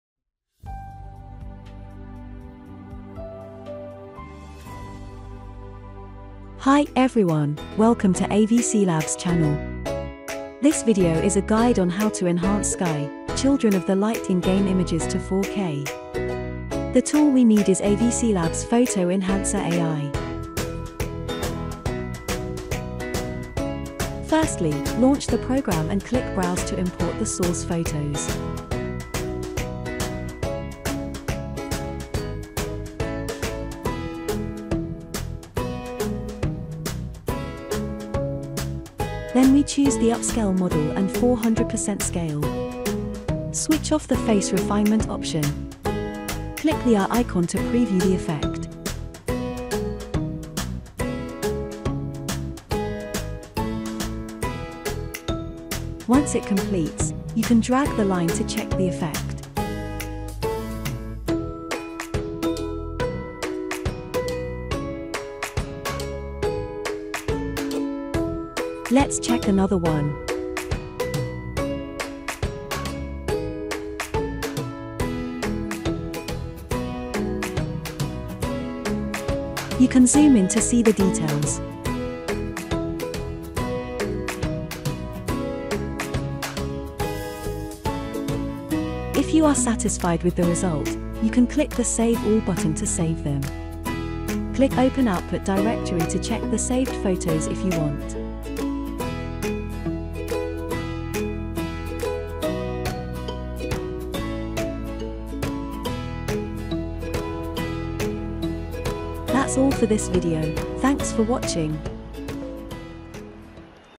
AI Enhance Sky: Children of the Light In-Game Images to 4K
You can upscale the beautiful pictures that you took in Sky to 4K wallpaper use AVCLabs Photo Enhancer AI

AVCLabs Photo Enhancer AI can optimize the photo quality, and make the old, blurred, or low-quality photos taken with old cameras or mobile phones to high-definition and clarity. Enlarge and upscale image without losing quality. For more info about the program, please

Steps to Enhance Sky In-Game Images:

1. Launch AVC Labs Photo Enhancer AI.
2. Click the Browse button to import photos.
3. Choose Upscale Model, 400% Scale and click the eye icon to preview the effect.
4. Then choose a photo to preview the effect.
5. Click Save All button to save these photos to your computer.